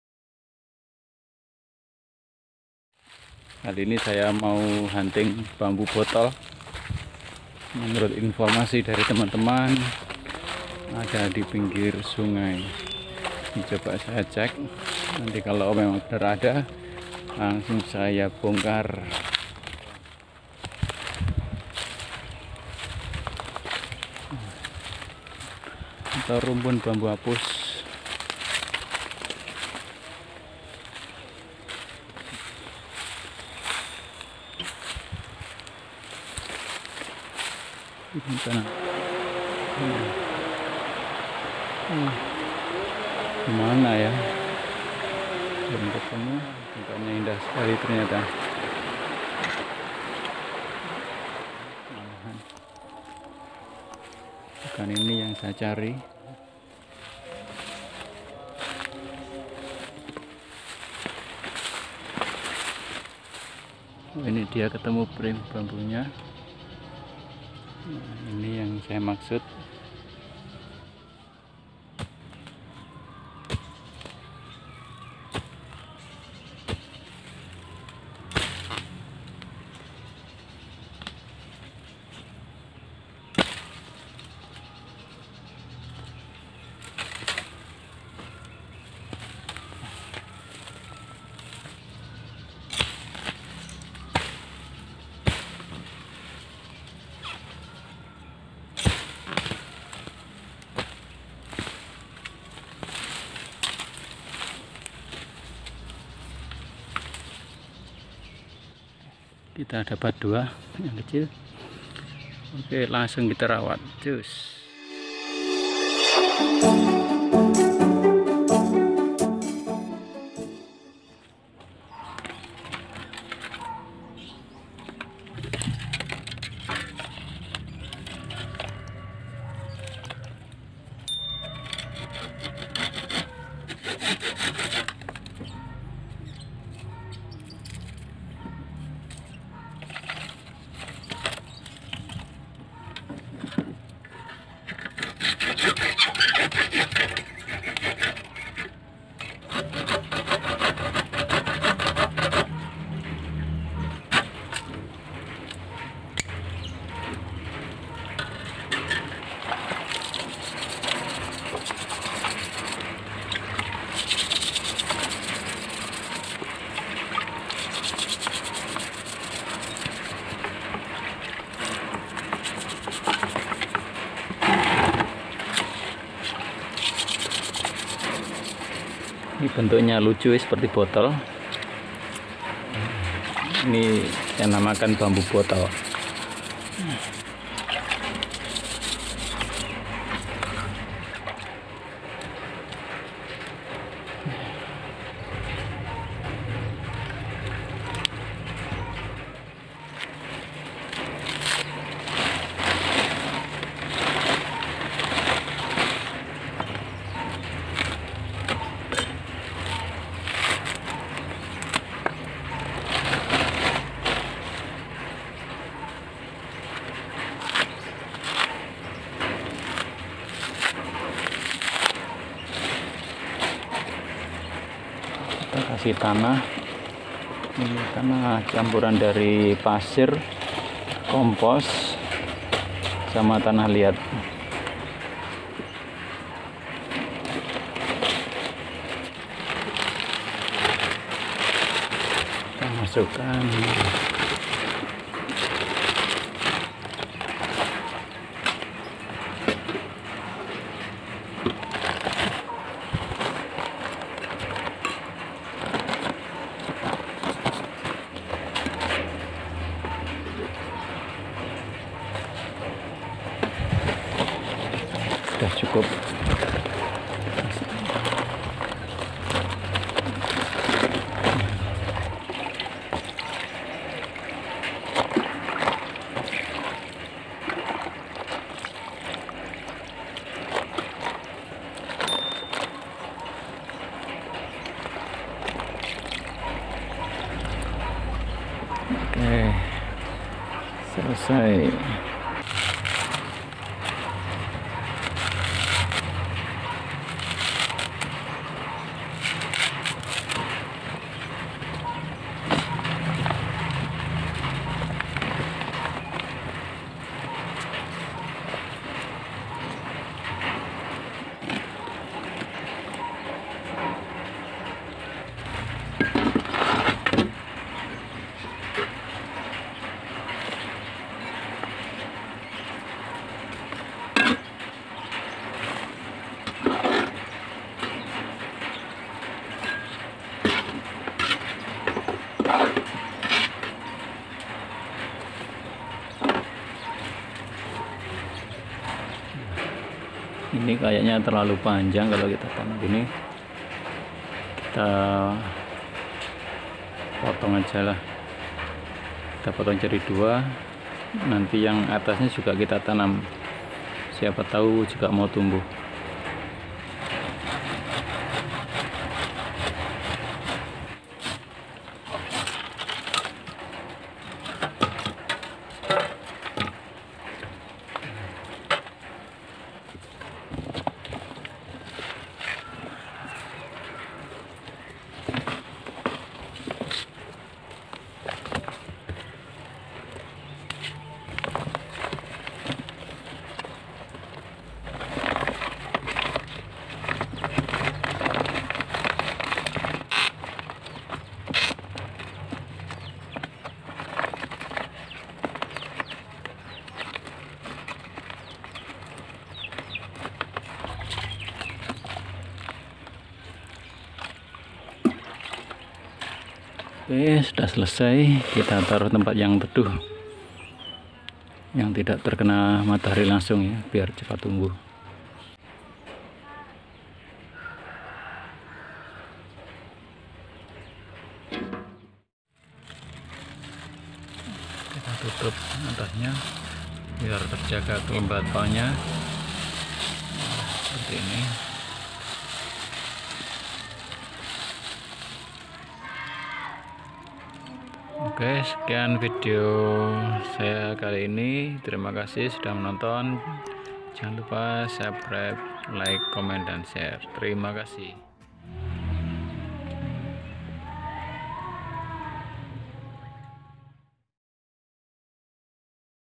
KOLEKSI JENIS BAMBU || BERBURU BAMBU BOTOL
Bambu ini bentuk aslinya sudah unik sangat bagus untuk dibuat hiasan dalam pot dan keberadaanya pun cukup langka sehingga saya termotivasi utk melestarikan dg cara menanam dlm bentuk bonsai2 bambu.
koleksi bambu#bambu hias#bambu botol#kerajinan bambu#bambu unik